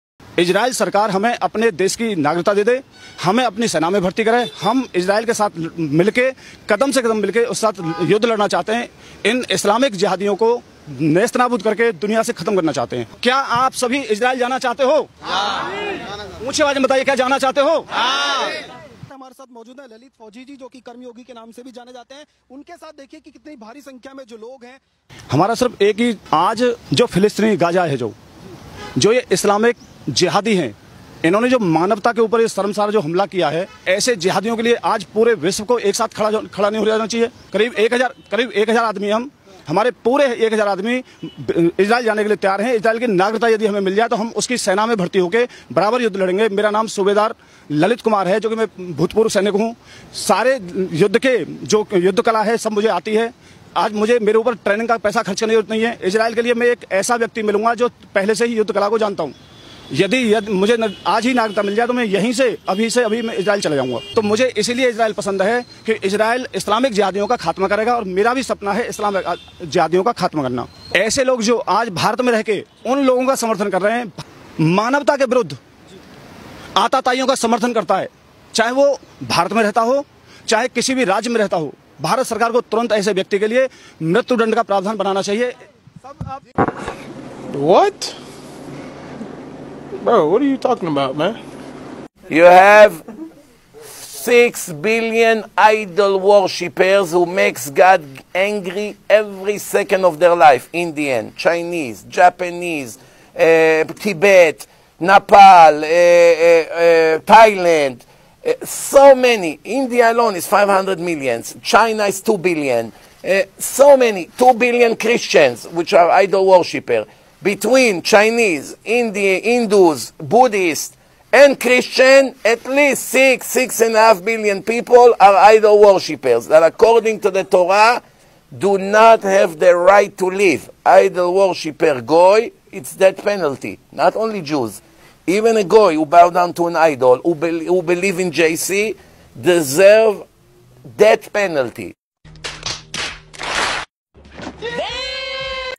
“Enlist Us To Fight In Gaza” Hindu Extremist Urge Israel. Rabbi Respond.
“Enlist Us To Fight In Gaza” Hindu Extremist Urge Israel. Watch the response from a Jewish Rabbi that will shock the Hindus.